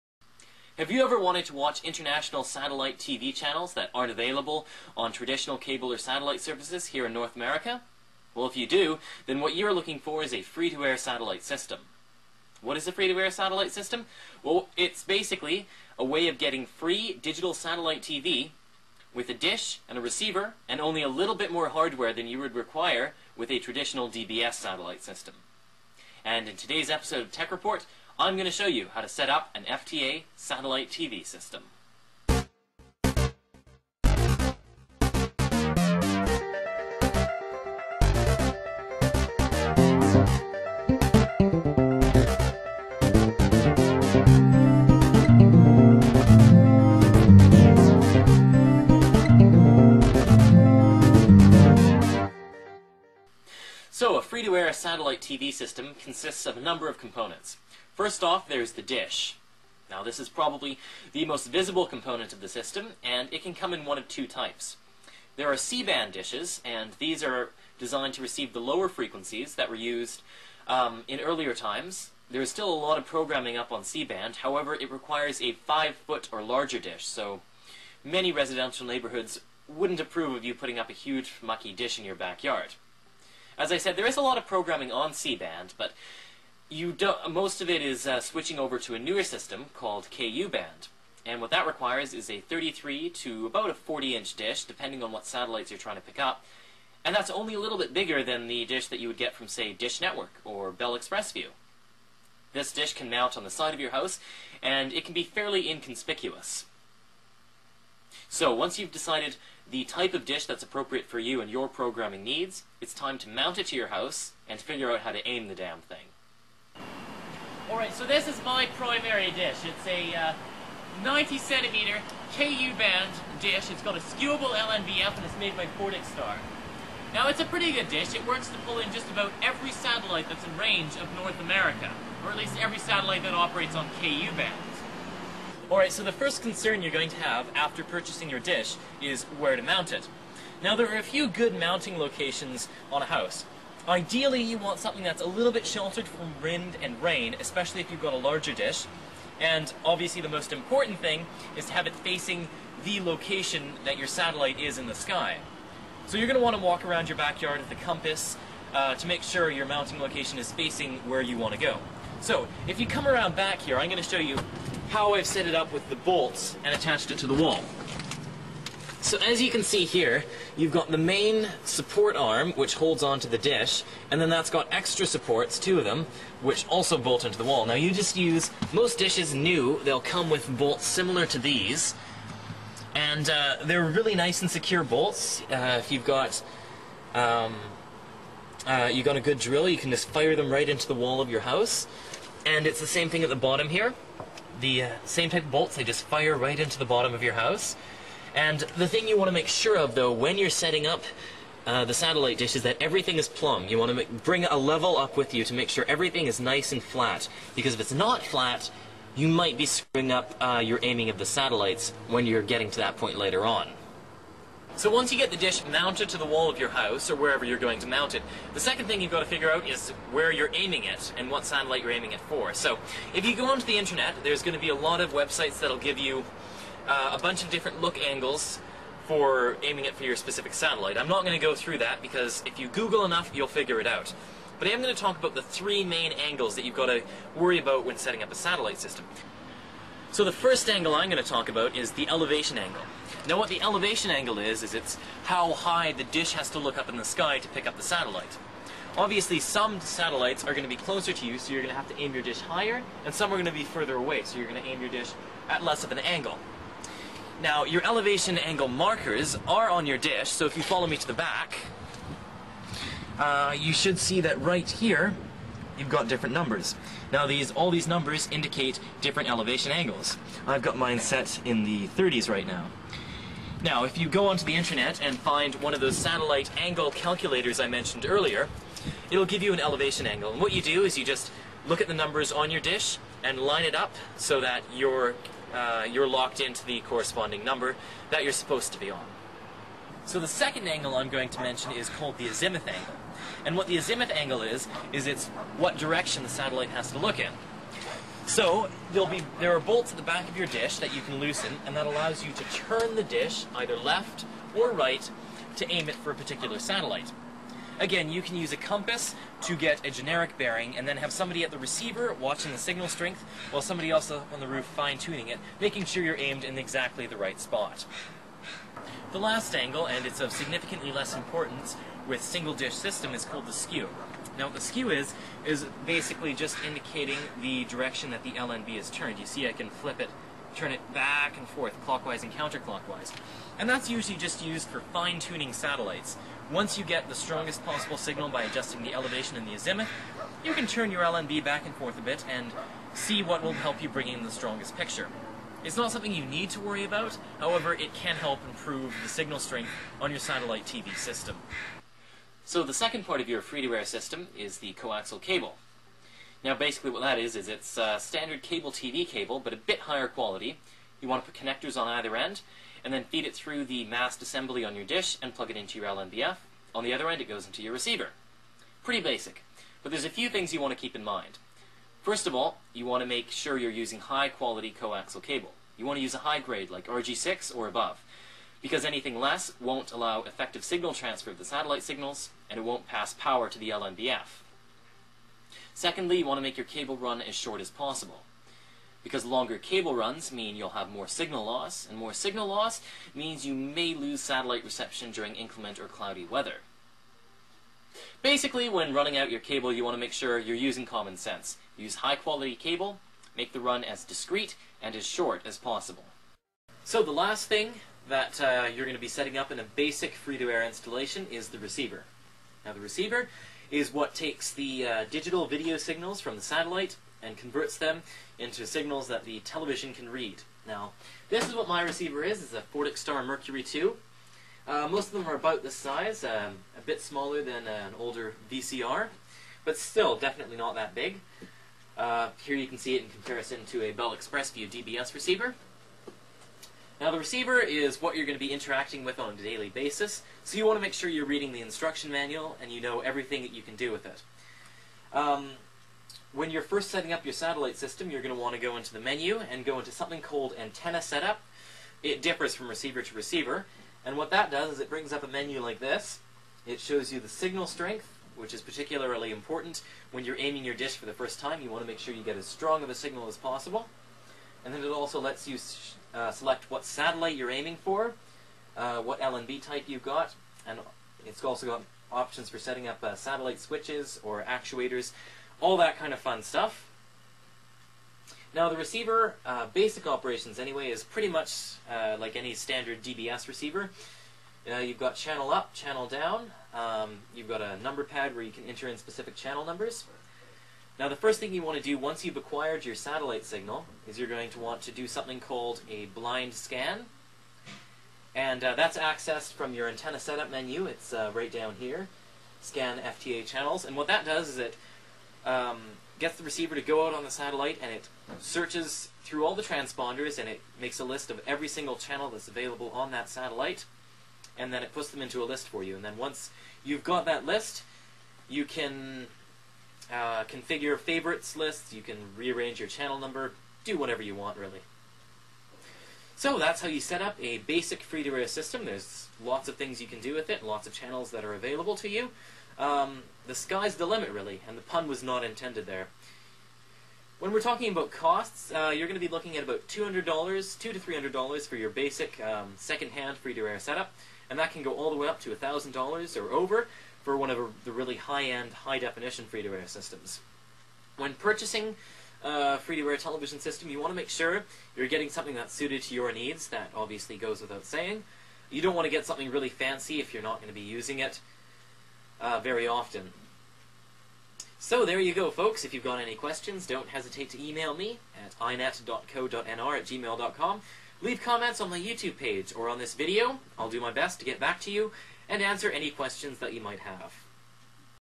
YouTube- How to Set up an FTA Satellite System.mp4
In this video I show the steps involved in setting up a legitimate FTA television system. I talk about elevation and azimuth angles, mounting locations, cable to use and receivers.